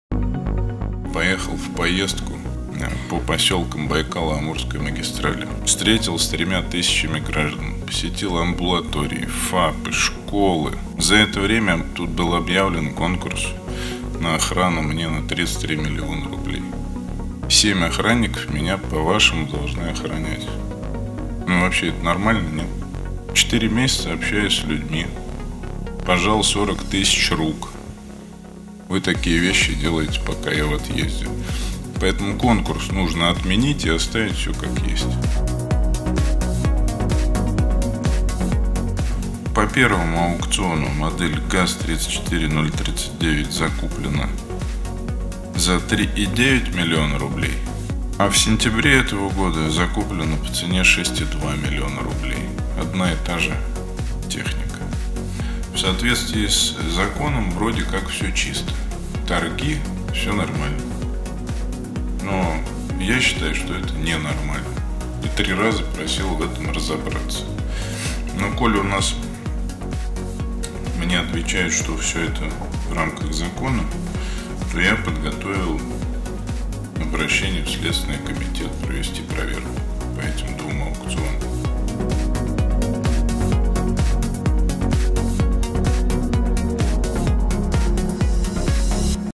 Михаил Дегтярёв раскритиковал систему госзакупок в Хабаровском крае
Михаил Дегтярёв раскритиковал систему госзакупок в Хабаровском крае. Глава региона выявил несколько сомнительных тендеров, профильные органы проводят проверку.

По мнению Дегтярёва, махинации с госзакупками позволяют заинтересованным лицам  заниматься хищением бюджетных средств. Часто одни и те же или почти одинаковые товары приобретаются правительством края по сильно разнящимся ценам. Так аудит закупки вездеходов показал, что комитет лесного хозяйства в 2020 году дважды покупал гусеничную технику (в июле и сентябре) у компаний, имеющих одного и того же учредителя. При этом цена закупок одинаковых вездеходов различалась почти в 2 раза. Изучив ситуацию, Дегтярёв принял решение обратиться в правоохранительные органы. На основании его обращения СК готовится провести проверку. Причём, по словам Дегтярёва, этот аукцион лишь один из череды подобных сомнительных закупок.

Михаил Дегтярёв уже не в первый раз демонстрирует, что при нём «тёмные делишки» в правительстве края не пройдут. Глава региона лично погружается во многие процессы, активно общается с населением, чтобы понимать проблемы региона изнутри и принимать решения, которые действительно пойдут на пользу краю.